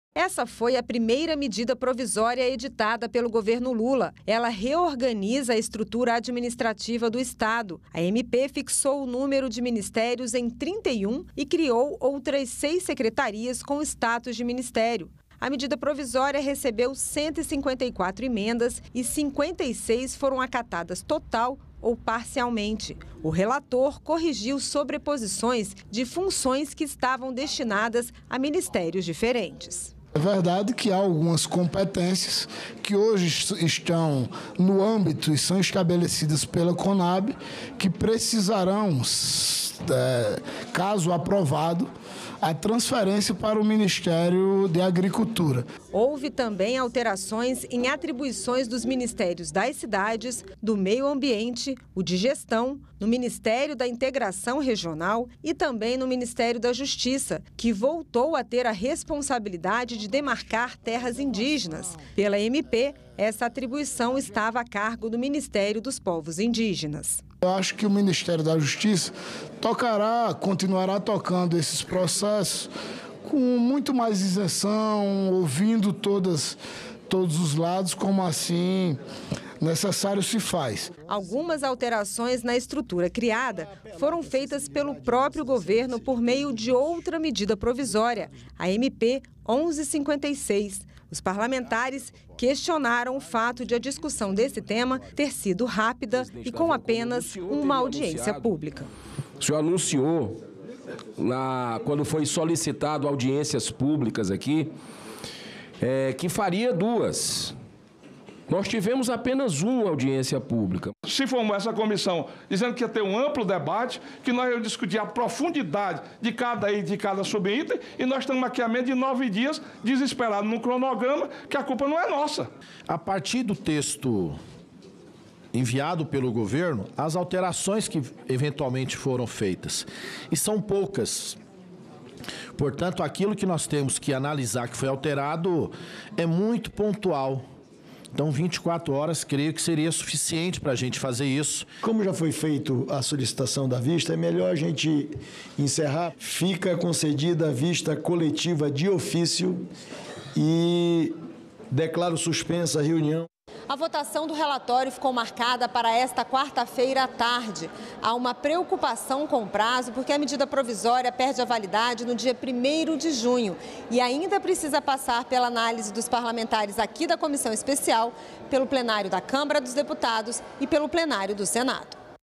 MP da Reestruturação dos Ministérios: relatório está pronto para votação
Ficou para esta quarta-feira (24), a votação do relatório sobre a medida provisória que reestrutura ministérios e outros órgãos do governo. Alguns parlamentares da Comissão Mista que analisa a proposta pediram tempo para estudar as mudanças no texto.